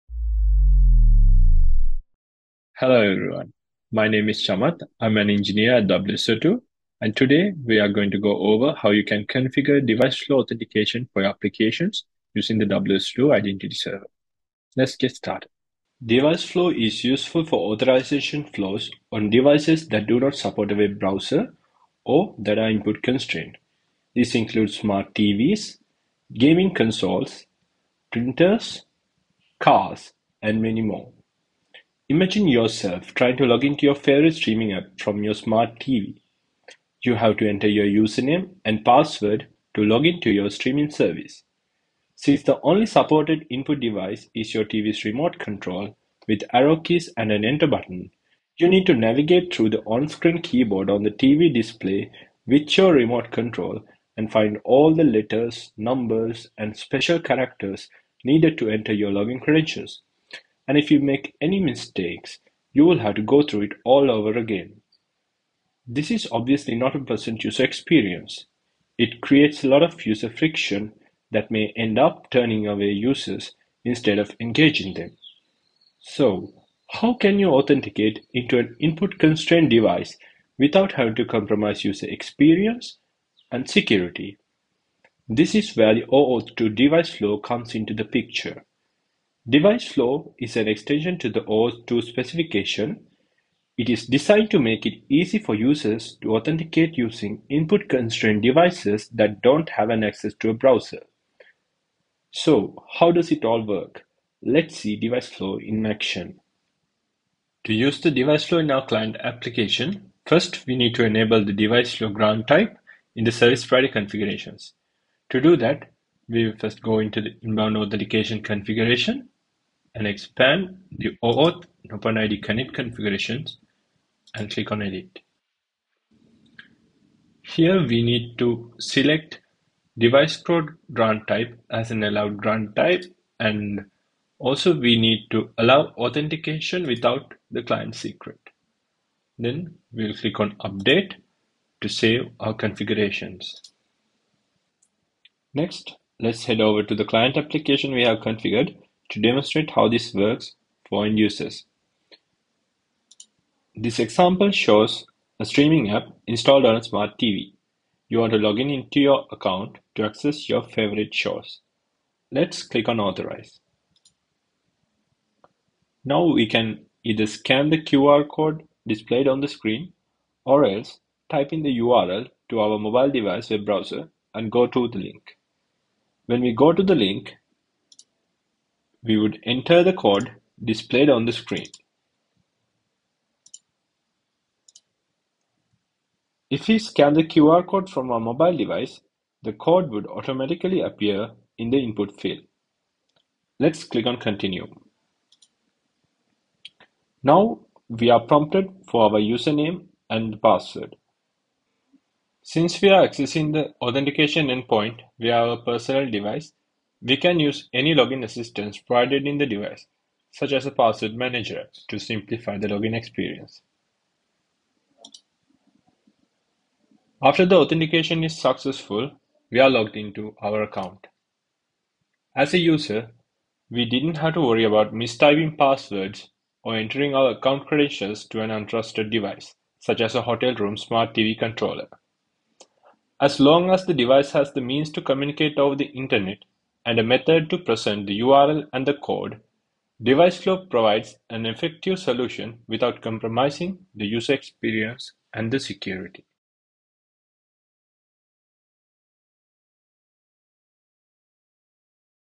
Device flow support with WSO2 Identity Server
Device flow is an OAuth2.0 extension that expands the range of devices an organization can use within their overall digital customer experience solution to include devices with limited user input capabilities (such as smart TVs which do not have keyboards). With device flow support, users can leverage other devices, such as smartphones, to complete the login on the limited input device.